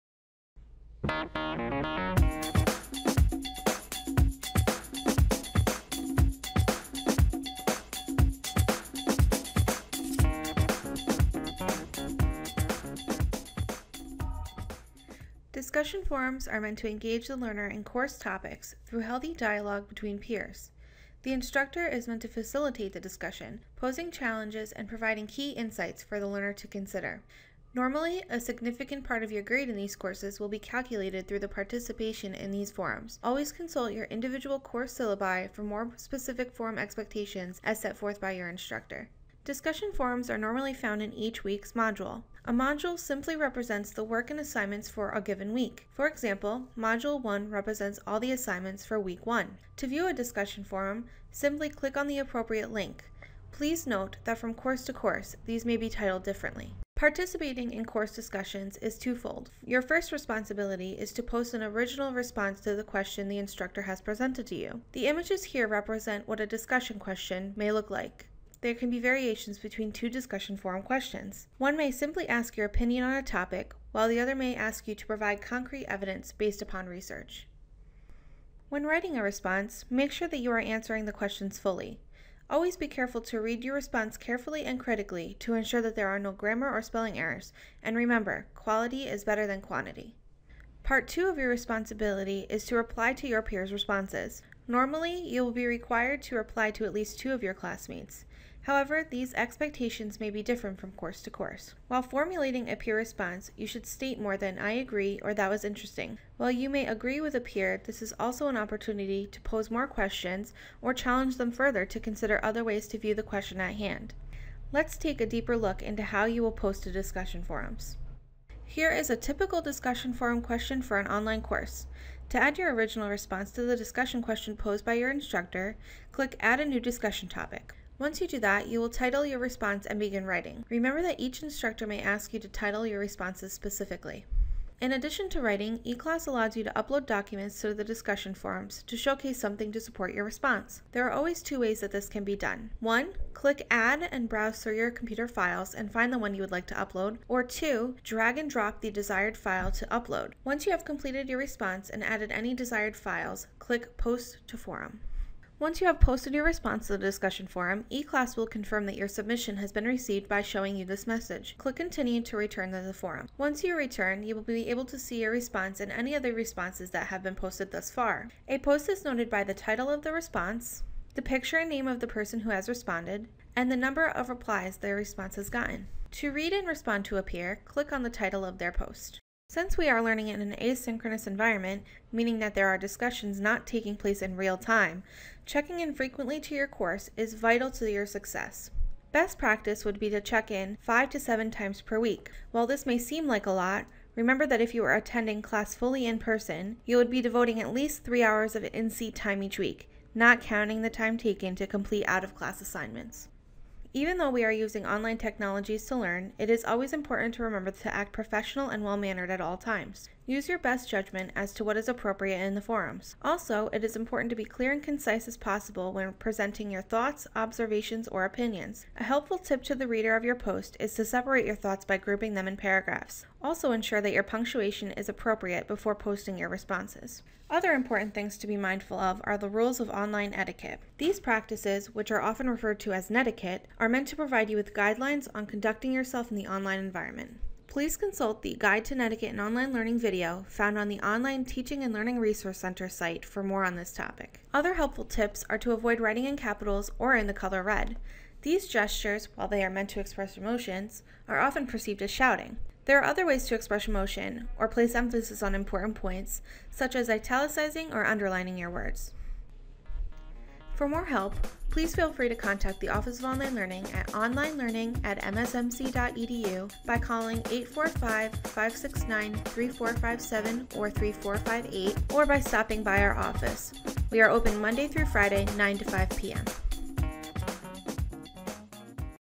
Student Resources: Discussion Forum Basics
This video provides online learners at Mount Saint Mary College a thorough look at participating in and the best practices of discussion forums.Visit onlinelearning.msmc.edu to view more best practices, tips for success, and video tutorials.